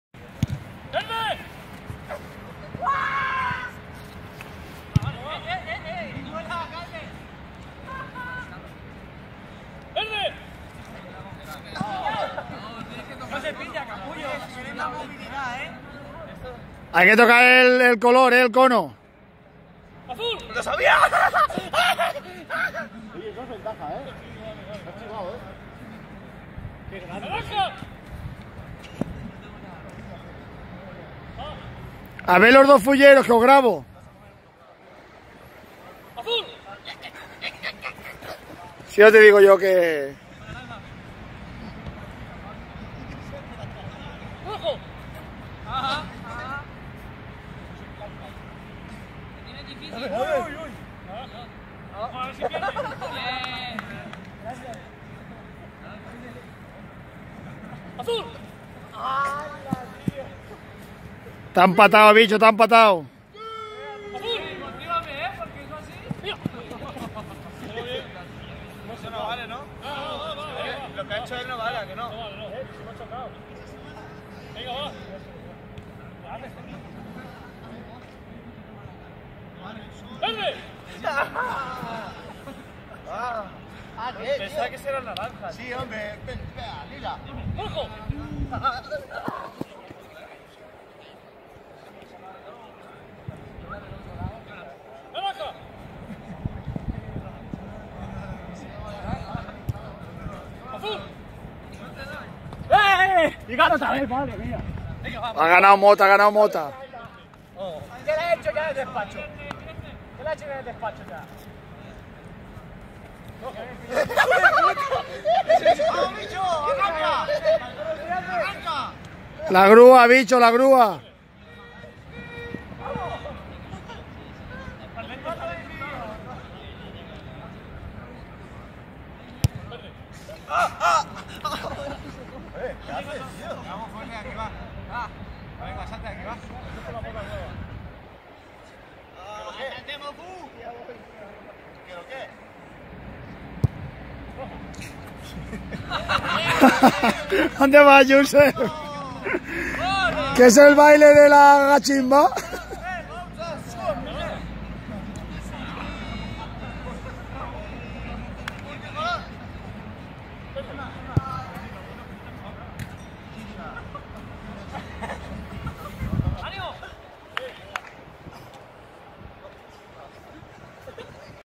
Juego de velocidad por David Serrano.
Juego de velocidad reacción por parejas y por colores.